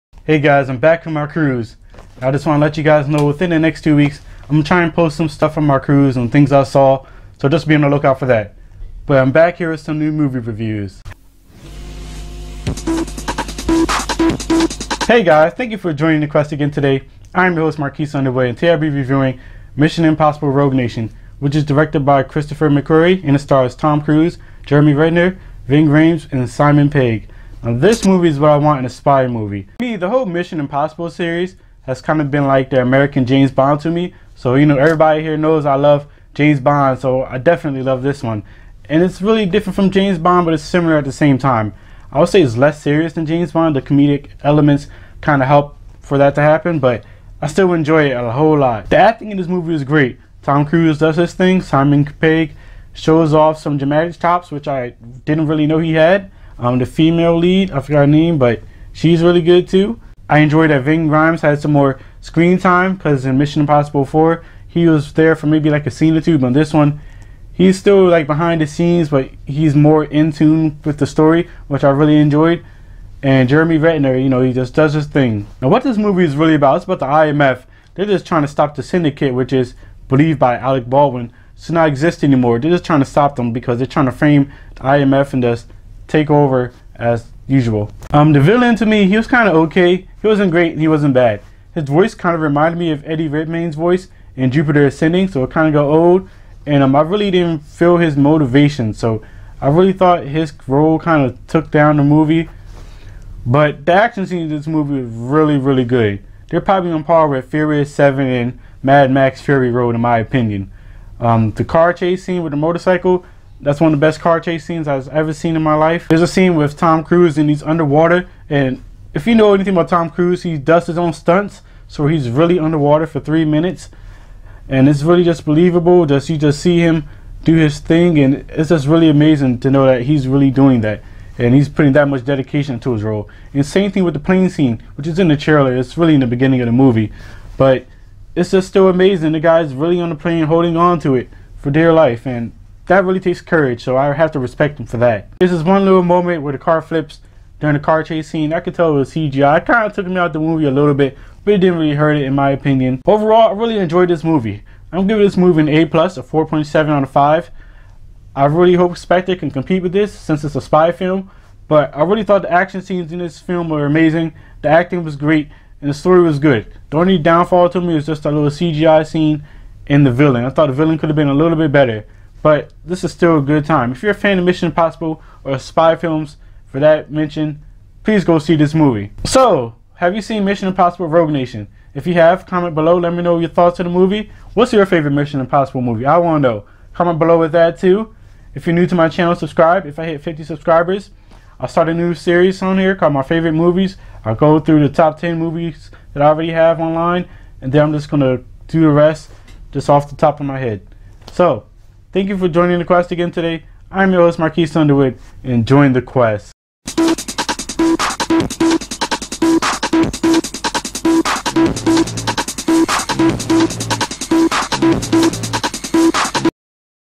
Mission Impossible: Rogue Nation Movie Review
Mission Impossible: Rogue Nation Movie Review by Marques Underwood

Tom Cruise is on a Mission. A Mission that is Impossible. A Rogue Nation is under the radar and he must stop them.